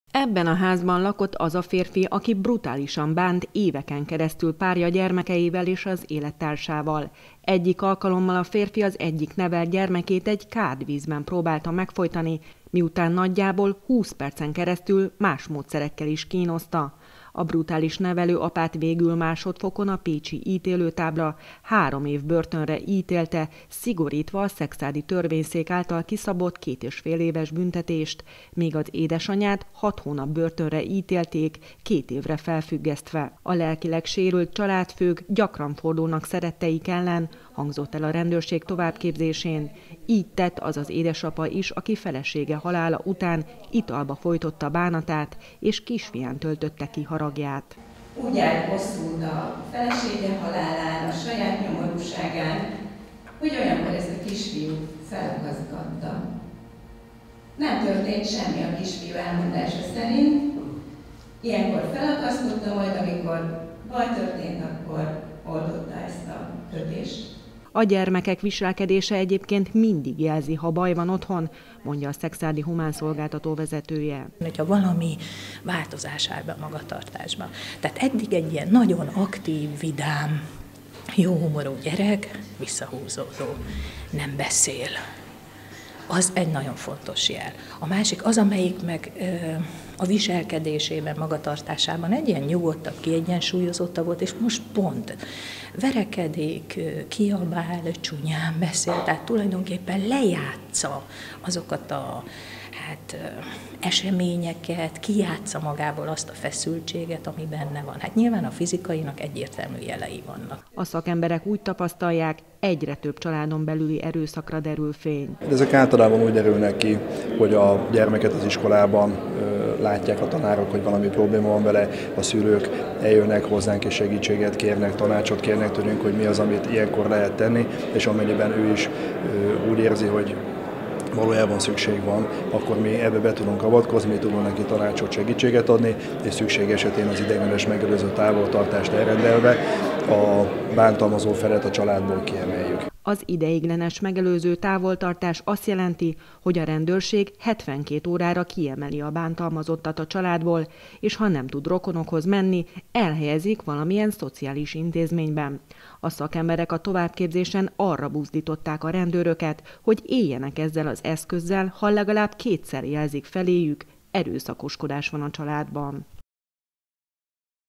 Családon belüli erőszak - egyre több esetre derül fény Tolna megyében 20181116
Egy édesapa rendszeresen felakasztotta kisfiát ittasan, mert nem tudta feldolgozni felesége halálát. Egy másik apa, ha ideges volt, rendszeresen bántotta nevelt gyermekeit. A rendőrség és a szociális szakma tapasztalatai szerint egyre több családon belüli erőszakra derül fény, aminek egyébként mindig vannak jelei.